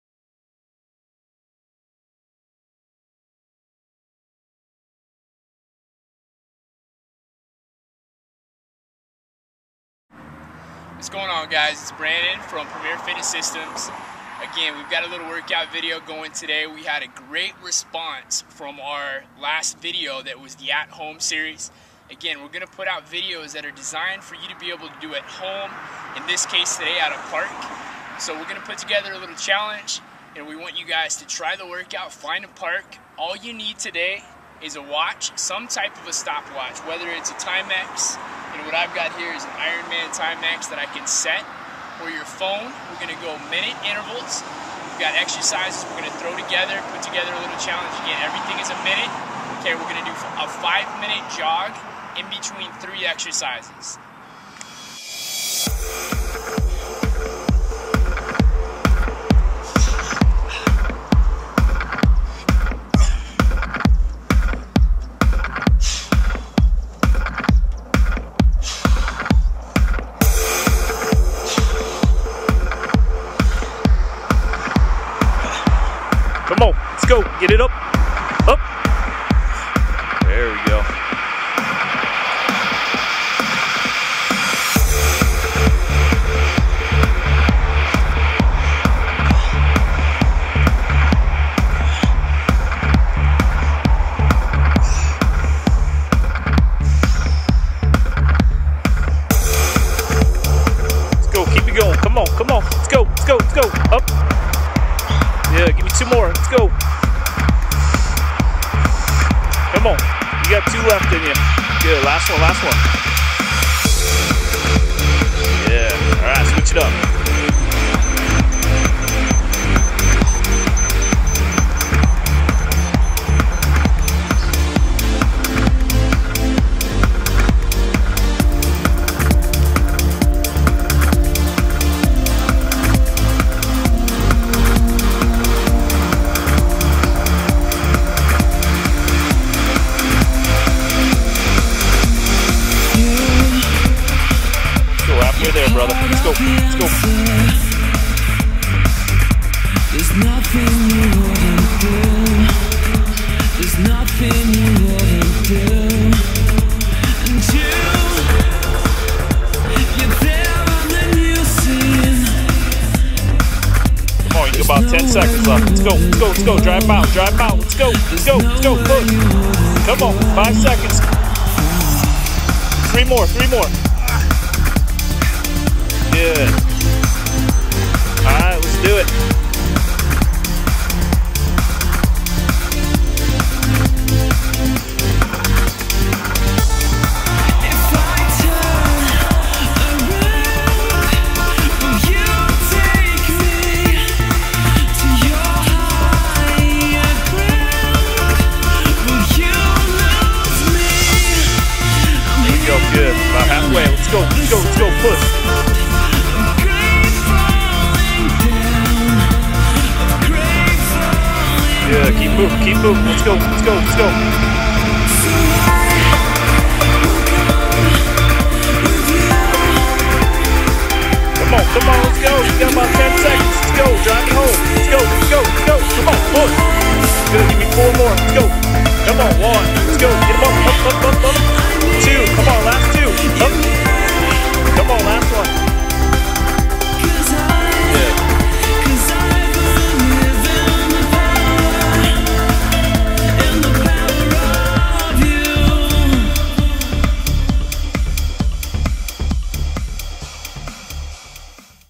Try If You Dare Ripped Park Workout
to see full description of the work out.
1min Max Muscle Ups or Pull Ups or Jump Pull ups
1min Max Push Ups or Knee Push Ups
1min Max Knee Ups
5min Jog
Repeat Circuit 3 Times